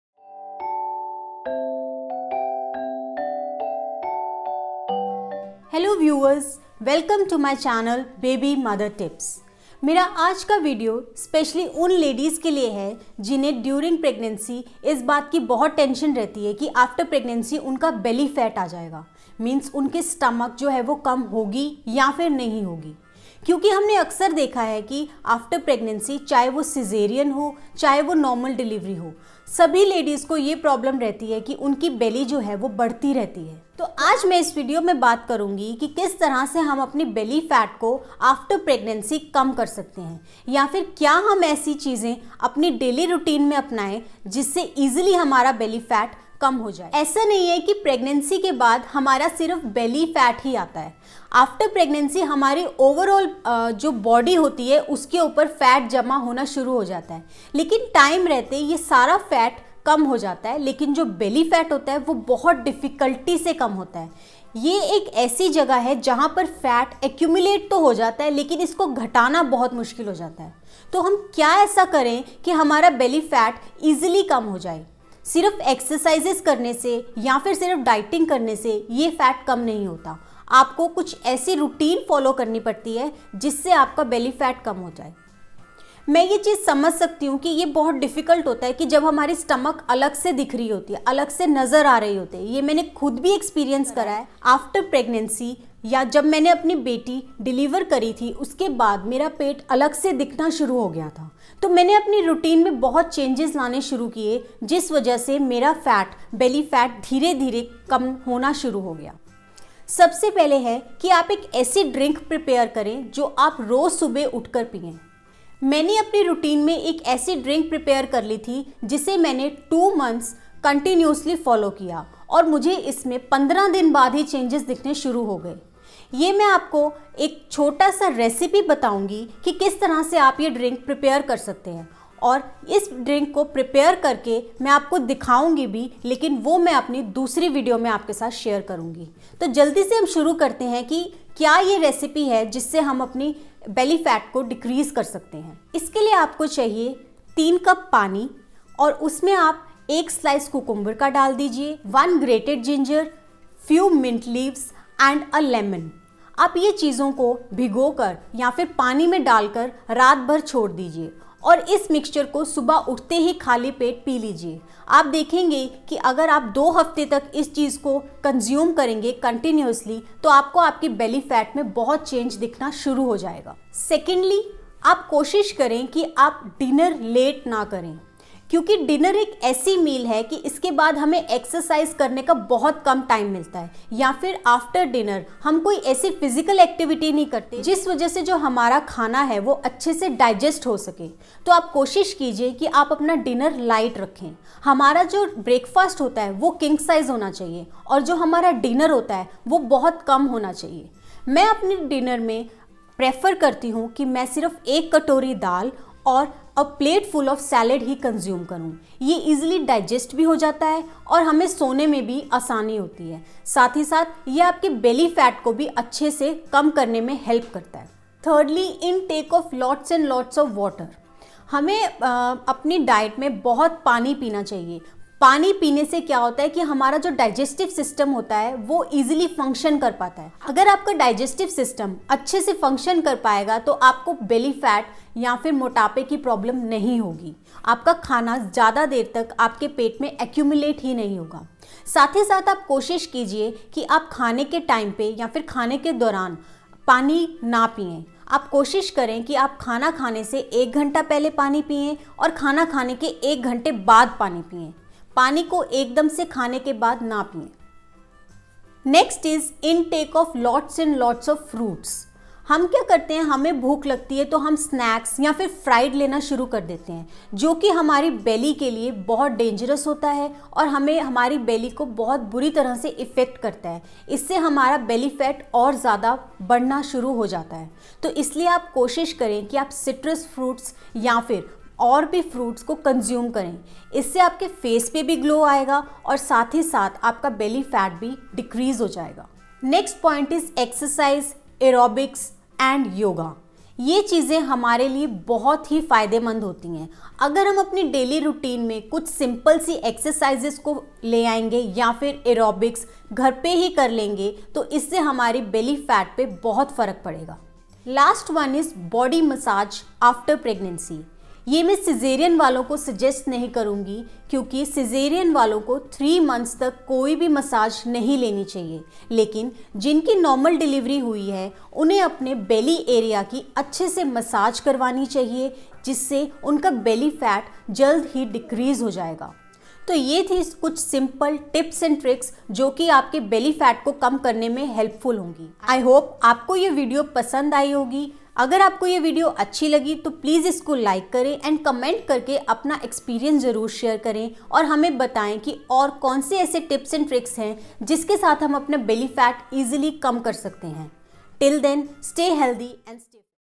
कैसे करें जल्दी Belly Fat कम? | Tips by Apoorva
Belly Fat कम करना बहुत ही challenging होता है especially pregnancy के बाद.  अपने इस video में मै कुछ ऐसी diets and exercises tips लायी हूँ जो कि Belly Fat reduce करने में बहुत effective हैं.  मेरी बेटी कि delivery के बाद मुझे भी belly fat हो गया था जो कि मैंने इन्ही tips कि help से बहुत कम समय में reduce कर लिया.

इस video को end तक ज़रूर देखें and मेरे दिए गए tips को diligently follow करें.  I am sure आप बहुत जल्दी results देखेंगे and यह आपके लिये बहुत ही helpful होगा.

Happy Parenting!